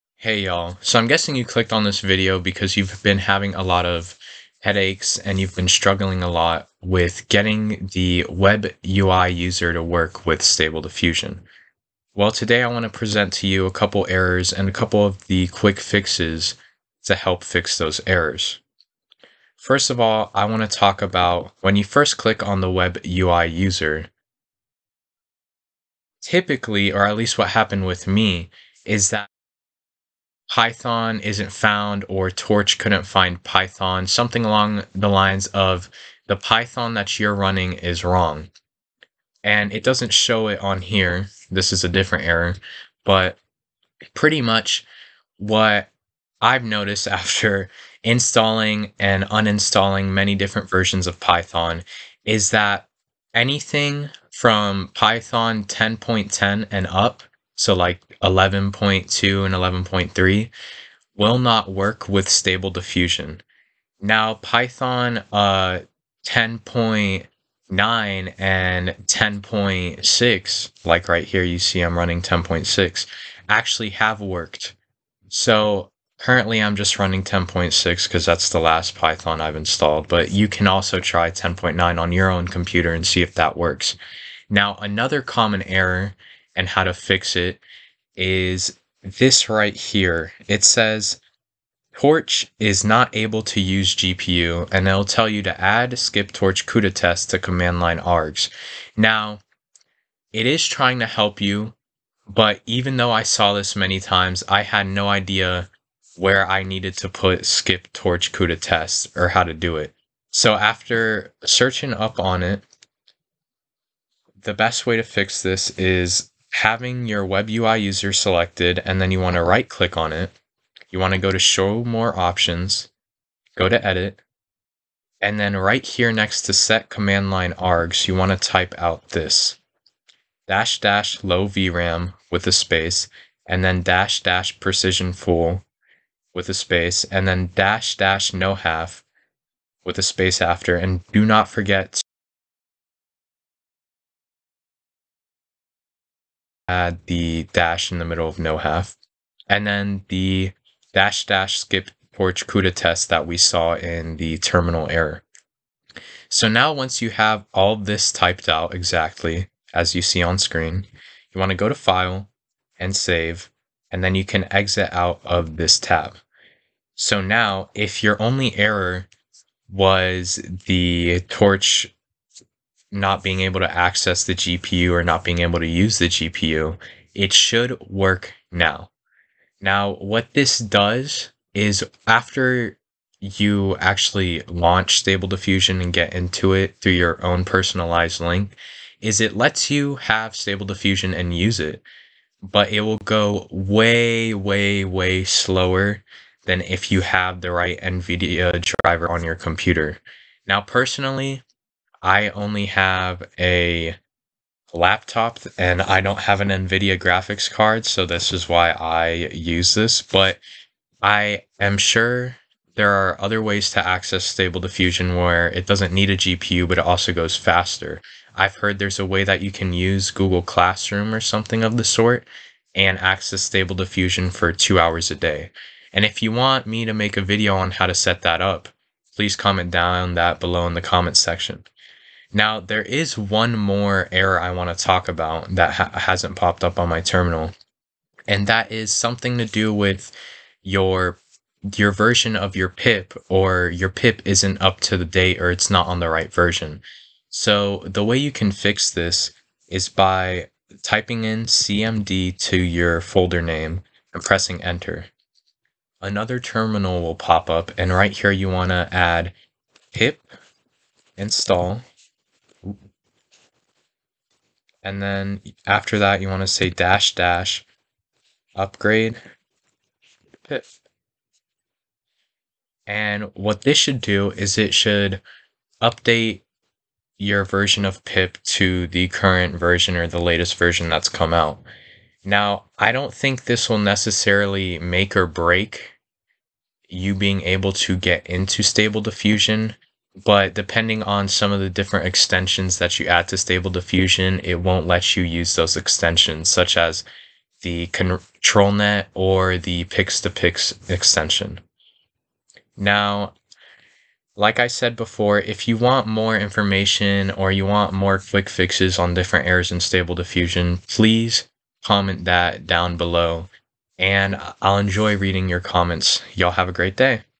Mastering Stable Diffusion: Common Errors and Easy Fixes
Learn how to fix common errors when setting up stable diffusion in this video. We'll cover hardware and software issues and provide quick fixes for each one. This video will help you find valuable tips and tricks to help you troubleshoot any problem that comes your way. By the end of this video, you'll have the knowledge and confidence to set up stable diffusion nice and easy.